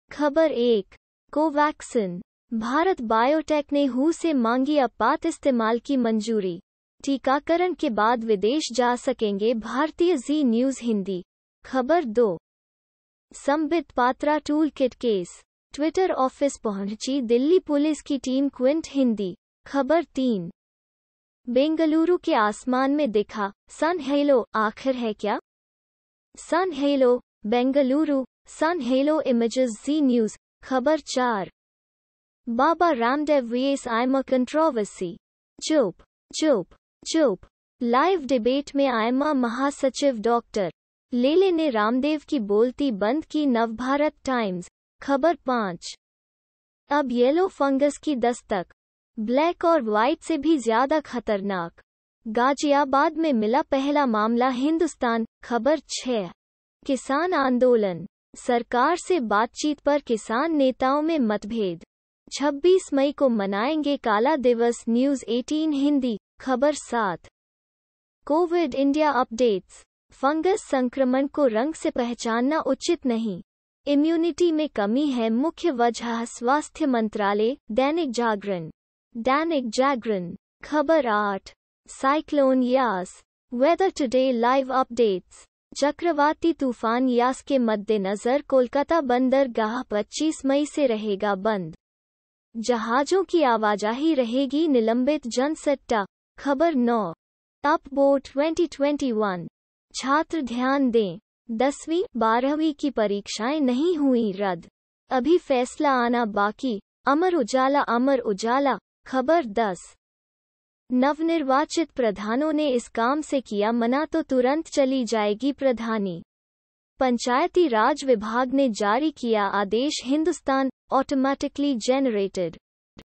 Hindi News (भारत)- 25/05/2021,12:00 AM
ख़बर 1:Covaxin: भारत बायोटेक ने WHO से मांगी आपात इस्तेमाल की मंजूरी, टीकाकरण के बाद विदेश जा सकेंगे भारतीय - Zee News Hindi, ख़बर 2:संबित पात्रा टूलकिट केस: ट्विटर ऑफिस पहुंची दिल्ली पुलिस की टीम - Quint Hindi, ख़बर 3:बेंगलुरु के आसमान में दिखा 'Sun Halo' आखिर है क्या? | Sun Halo | Bengaluru | Sun Halo Images - Zee News, ख़बर 4:Baba Ramdev vs IMA Controversy: चुप, चुप, चुप... लाइव डिबेट में IMA महासचिव डॉ. लेले ने रामदेव की बोलती बंद की - Navbharat Times, ख़बर 5:अब येलो फंगस की दस्तक, ब्लैक और वाइट से भी ज्यादा खतरनाक, गाजियाबाद में मिला पहला मामला - Hindustan, ख़बर 6:किसान आंदोलन: सरकार से बातचीत पर किसान नेताओं में मतभेद, 26 मई को मनाएंगे काला दिवस - News18 हिंदी, ख़बर 7:Covid India Updates: फंगस संक्रमण को रंग से पहचानना उचित नहीं, इम्यूनिटी में कमी है मुख्य वजहः स्वास्थ्य मंत्रालय - दैनिक जागरण (Dainik Jagran), ख़बर 8:Cyclone Yaas, Weather Today Live Updates: चक्रवाती तूफान यास के मद्देनजर कोलकाता बंदरगाह 25 मई से रहेगा बंद, जहाजों की आवाजाही रहेगी निलंबित - Jansatta, ख़बर 9:Up Board 2021 : छात्र ध्यान दें, 10वीं-12वीं की परीक्षाएं नहीं हुईं रद्द, अभी फैसला आना बाकी - अमर उजाला - Amar Ujala, ख़बर 10:नवनिर्वाचित प्रधानों ने इस काम से किया मना तो तुरंत चली जाएगी प्रधानी, पंचायती राज विभाग ने जारी किया आदेश - Hindustan. Automatically Generated... For Advertisement Contact